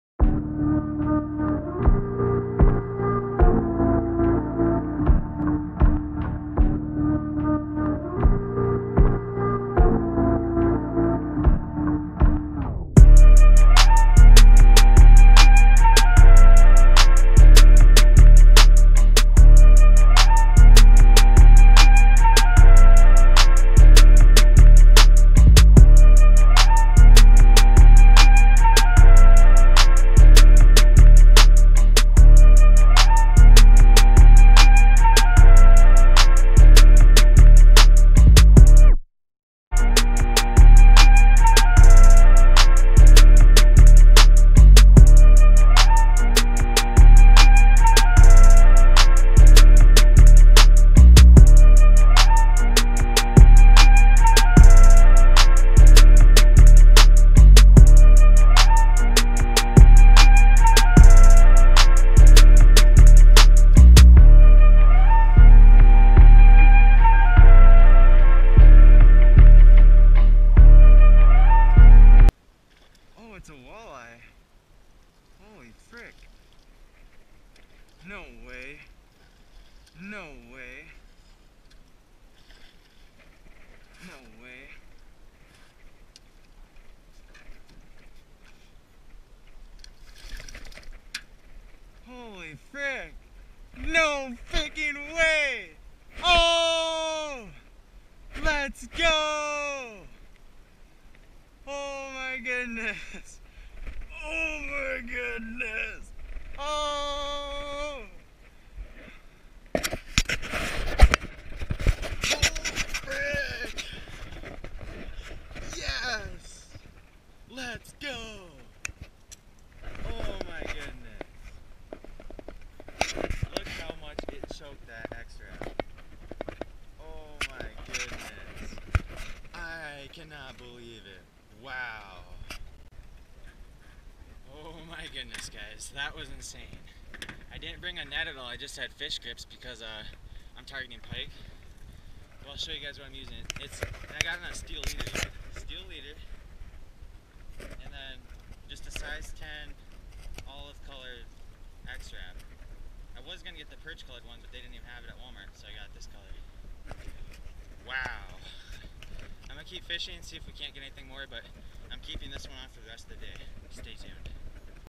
Catching a 3 lb Walleye Using Rapala X-Rap in Pressured Waters!
Fishing for pike when I hooked into this beast!